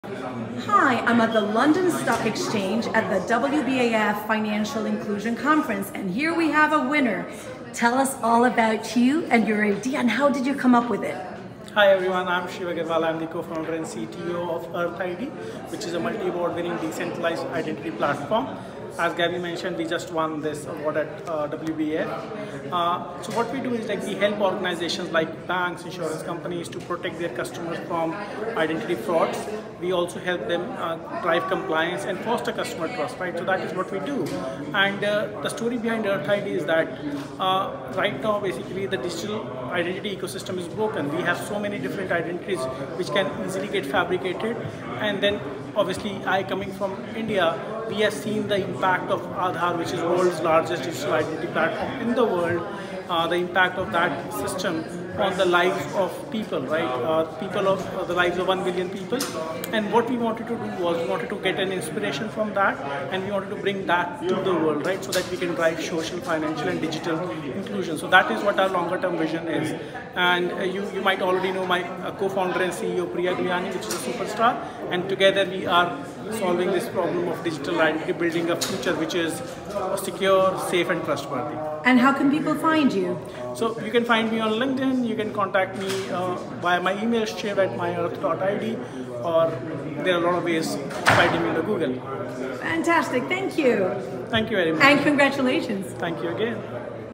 Shiv Aggarwal, Co-Founder and CTO at Earth ID wins pitch at WBAF conference at the LSE Launch
Shiv Aggarwal, Co-Founder and CTO of Earth ID wins pitch at WBAF Financial Inclusion Conference at the London Stock Exchange on May 7th.